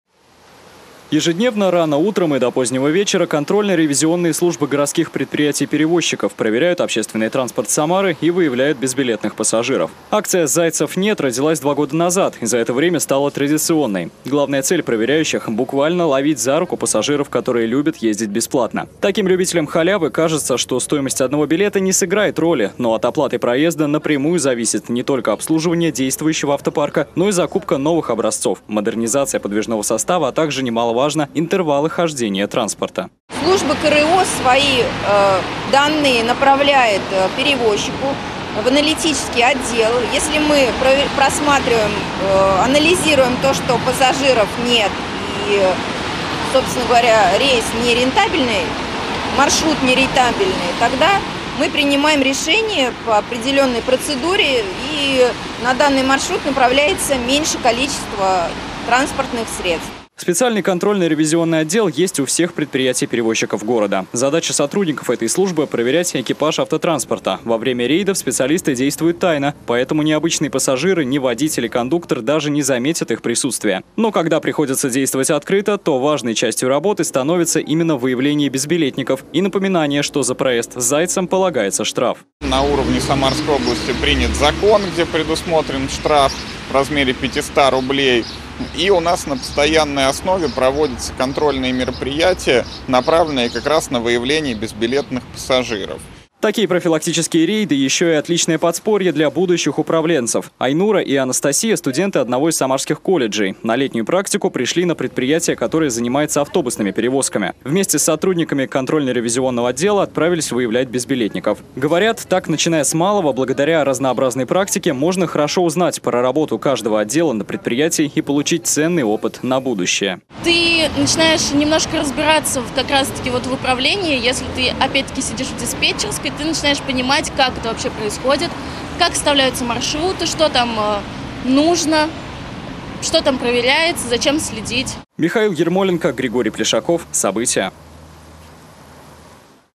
В Самаре прошел рейд по выявлению безбилетников в общественном транспорте
Без билета нет проезда. В общественном транспорте Самары продолжают ловить любителей ездить бесплатно. Контрольно-ревизионная группа проводит профилактические рейды "Зайцев нет". Почему важно оплачивать каждую поездку и в чем особенности работы проверяющих расскажет Михаил Ермоленко.